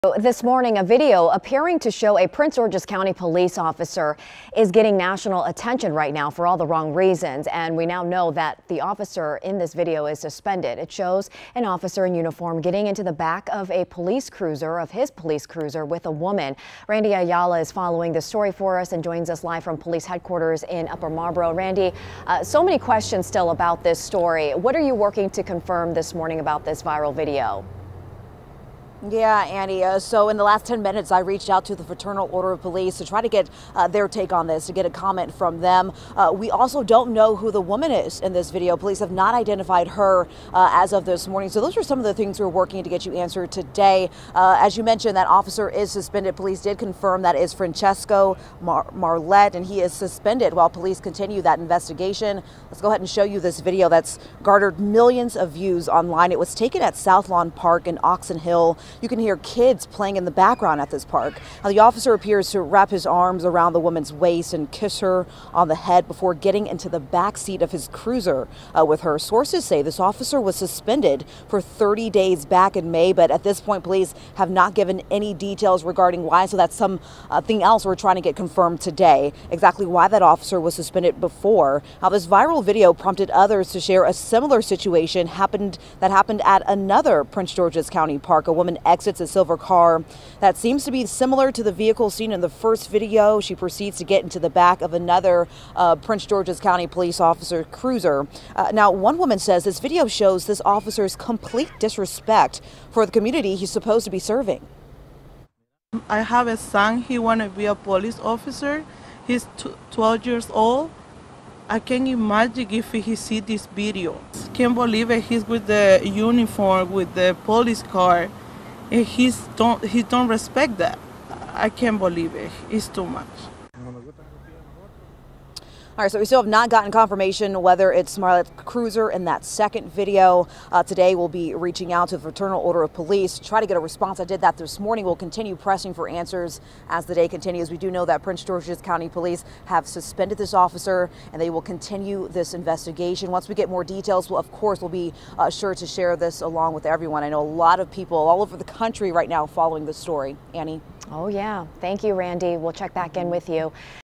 Officer suspended after viral video prompts investigation in Prince George's County
The video is getting national attention for all the wrong reasons.